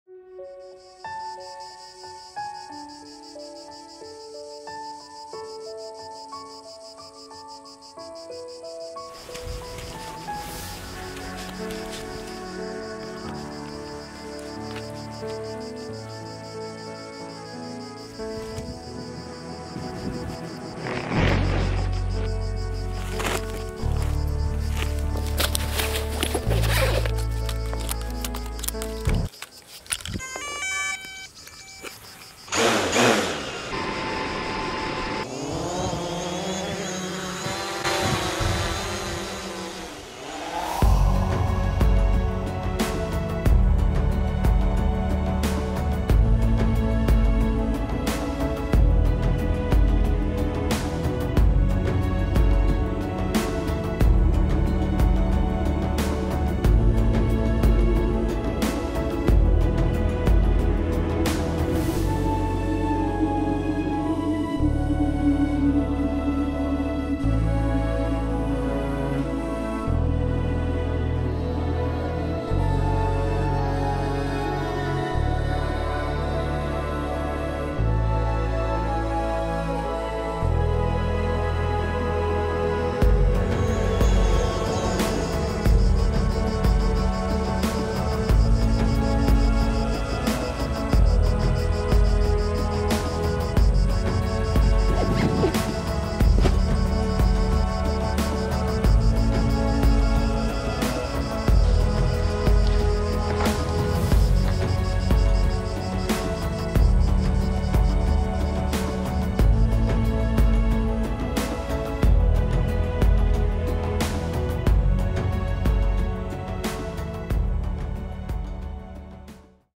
The Last Day of Summer - A short film Mavic Pro + Canon D80 + Canon G7XmII
A short film recorded and edited for online training purposes. The point was to give some idea of showing the aerial footage.
The Production is dedicated to my online students.
I really hope you guys enjoy this film! I will be recording a new online training in the weeks to come. Leave any questions or things you’d like to hear about below!

Thank you for your support, and I hope you guys enjoyed the film!